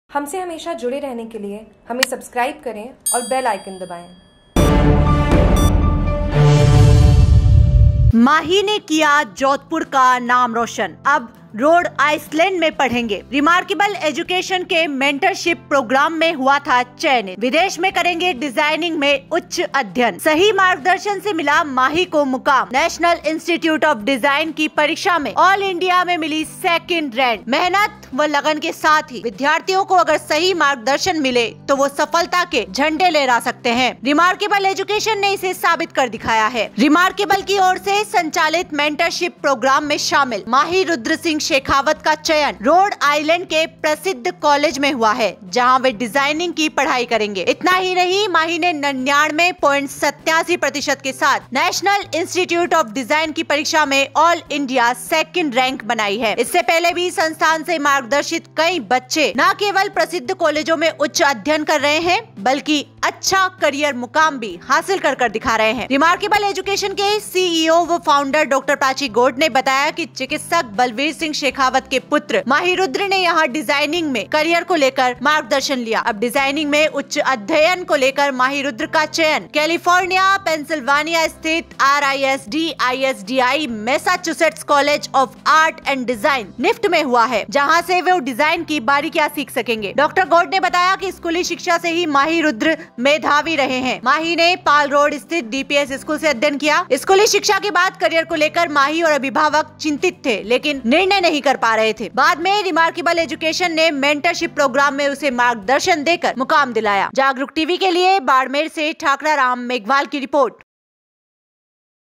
बाडमेर : माही ने किया जोघपुर का नाम रोशन, अब रोड आइसलैंड में पढ़ेगा
सुर्य नगरी   कर दिखाया मेहनत व लगन के साथ ही विद्यार्थियों को अगर सही मार्गदर्शन मिले तो वे सफलता के झंडे फहरा सकते हैं। रिमॉर्केबल एजुकेशन ने इसे साबित कर दिखाया है। रिमॉर्केबल की ओर से संचालित मेंटरशिप प्रोगाम में शामिल माहीरुद्र सिंह शेखावत का चयन  रोड आइसलैंड के प्रसिद्ध कॉलेज में हुआ है, जहां वे डिजाइनिंग की पढ़ाई करेंगे।
इतना ही नही, माही ने 99.87 प्रतिशत के साथ नेशनल इंस्टीट्यूट ऑफ डिजाइन की परीक्षा में ऑल इंडिया सेकेंड रैंक बनाई है। इससे पहले भी संस्थान से मार्गद​र्शित कई बच्चे न केवल प्रसिद्ध कॉलेजों में उच्च अध्ययन कर रहे हैं ब​ल्कि अच्छा कॅरियर मुकाम भी हासिल कर दिखाया है। रिमॉर्केबल एजुकेशन की सीईओ व फाउंडर डॉ. प्राची गौड़ ने बताया कि चिकित्सक बलवीर सिंह शेखावत के पुत्र माहीरुद्र ने यहां डिजाइनिंग में कॅरियर को लेकर मार्गदर्शन लिया। अब डिजाइन में उच्च अध्ययन को लेकर माहीरुद्र का चयन  कैलिफोर्निया पेन्सिलवानिया स्थित आरआईएसडी, आईएसडीआई, मैसाचुसेट्स कॉलेज ऑफ आर्ट एंड डिजाइन, निफ्ट में हुआ है। जहां से वे डिजाइन की बारीकियां सीख सकेंगे। डॉ. गौड ने बताया कि स्कूली ​शिक्षा से ही माहीरुद्र मेधावी रहा है। माही ने पाल रोड स्थित डीपीएस स्कूल से अध्ययन किया। स्कूली ​शिक्षा के बाद कॅरियर को लेकर माही व अ​भिभावक चिंतित थे और निर्णय नहीं ले पा रहे थे। बाद में रिमॉर्केबल एजुकेशन ने मेंटर​शिप प्रोगाम में उसे मार्गदर्शन देकर मुकाम दिलाया